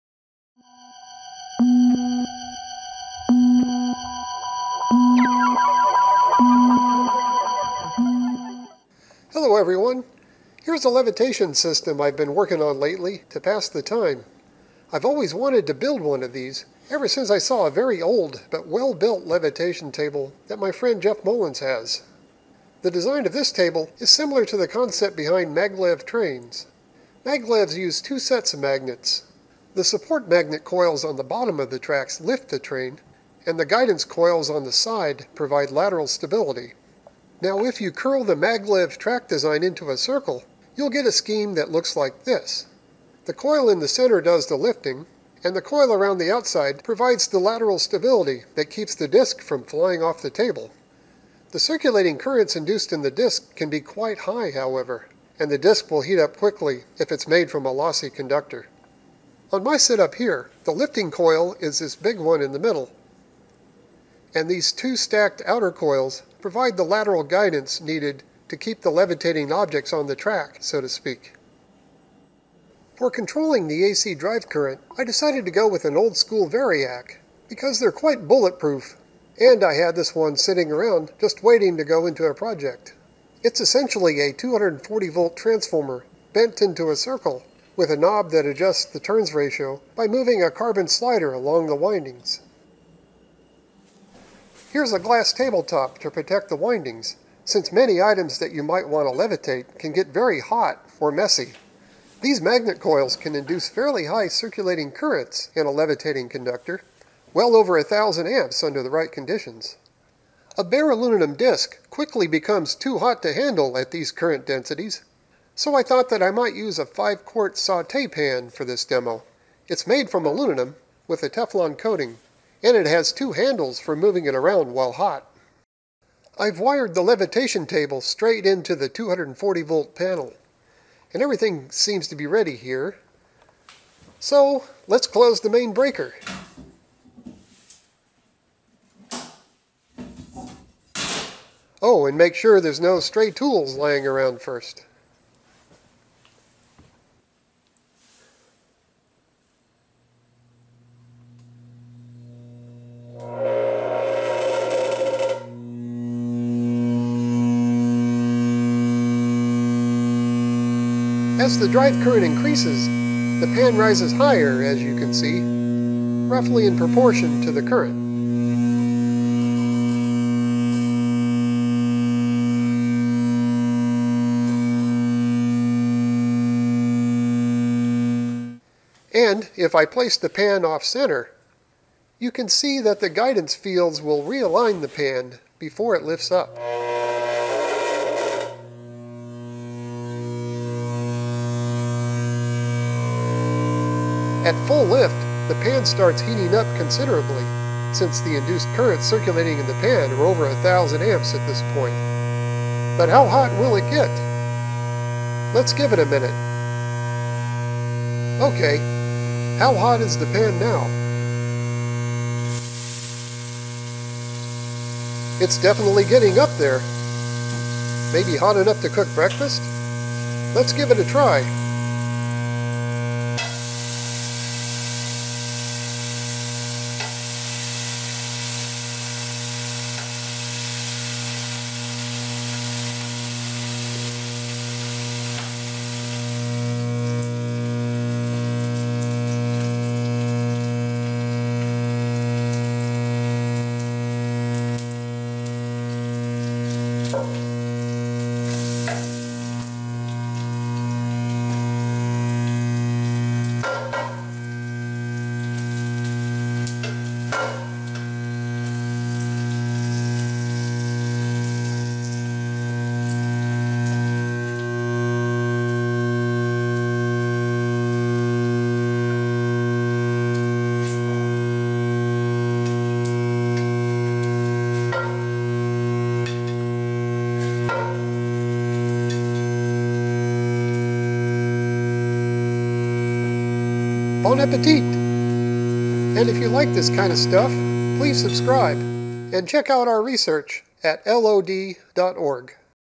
Levitation
Using induced currents and Lenz's Law to levitate objects.